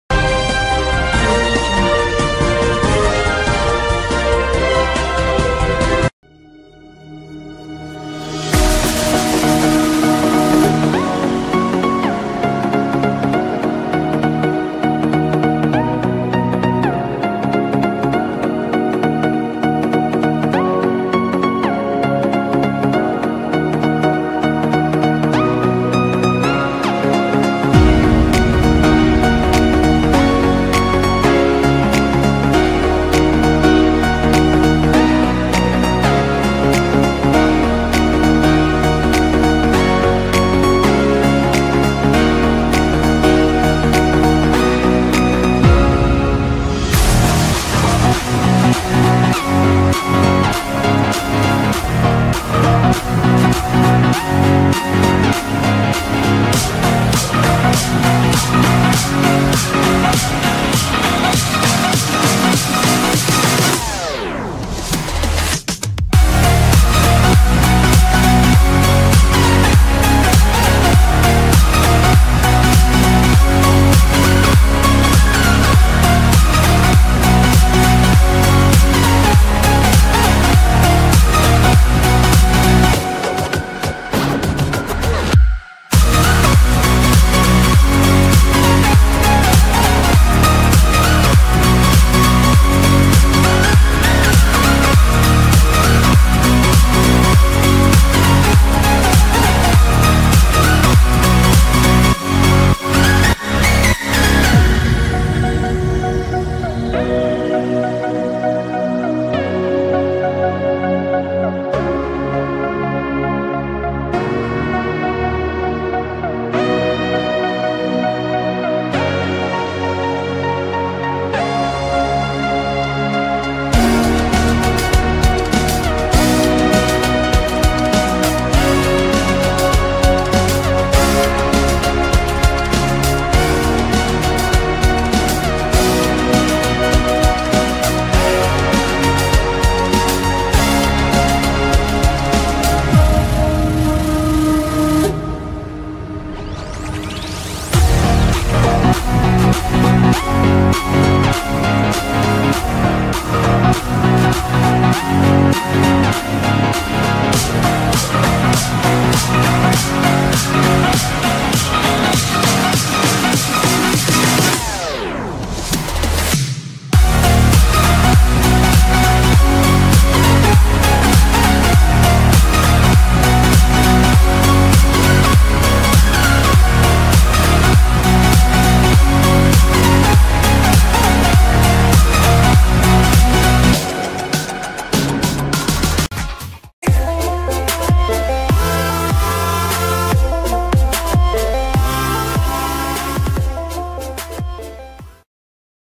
Liverpool transfer news: Real Madrid make Marco Asensio decision amid Reds interest
► The winger has been linked with a move to Liverpool in recent weeks, with Real Madrid preparing for ...
► PLL:
► Photo & Content Source :